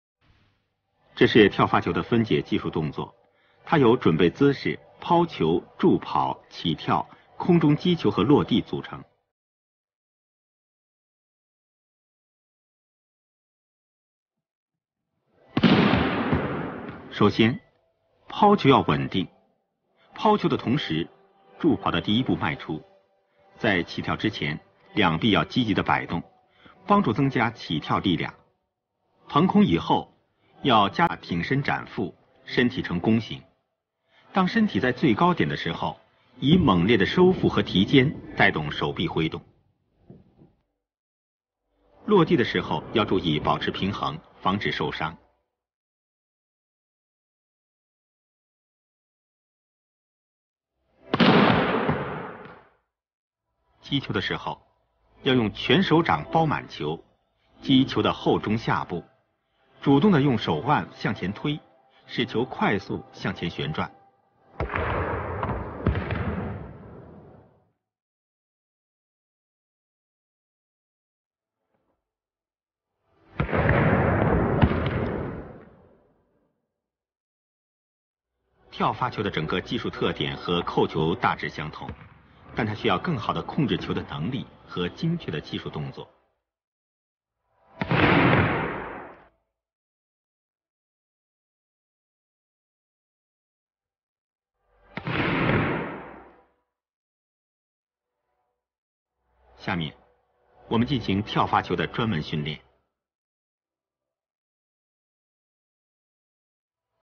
Basics Of JUMP SERVE In VOLLEYBALL SLOW MOTION
How to jump serve? VERY EASY!!! After Watching This Video I m Sure That You Will Be A better Server